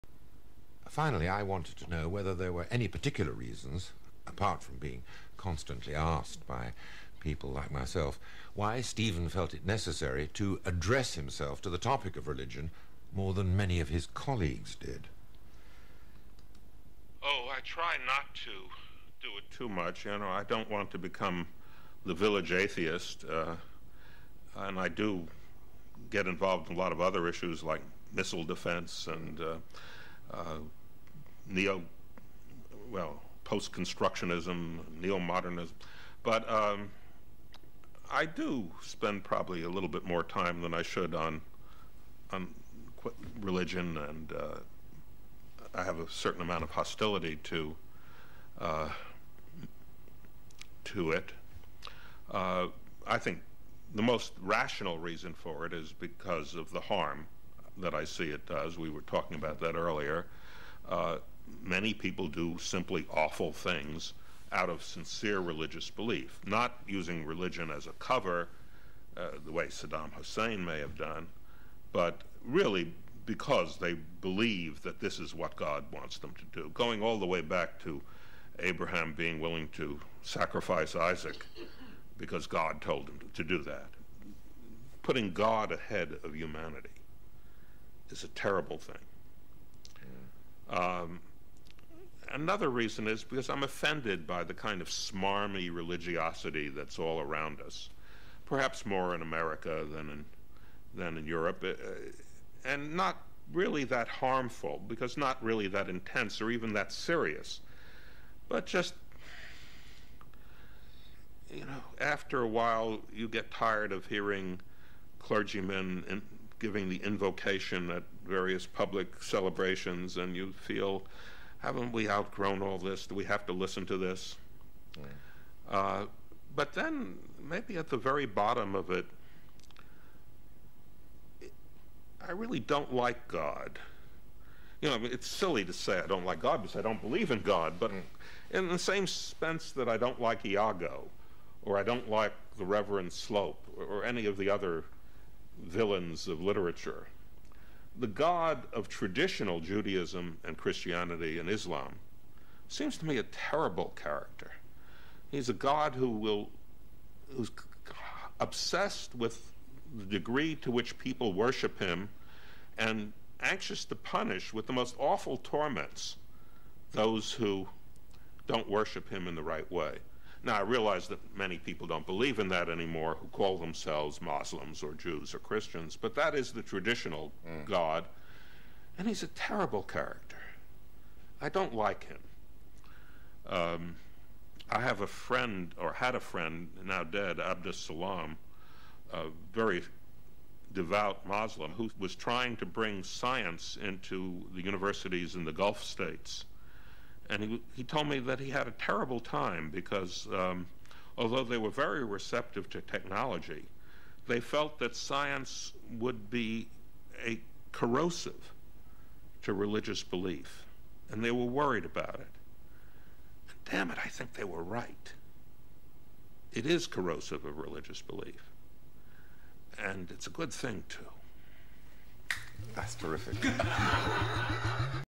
Steven Weinberg on his anti-theism
The nobel prize winning physicist Steven Weinberg talks to Jonathan Miller about atheism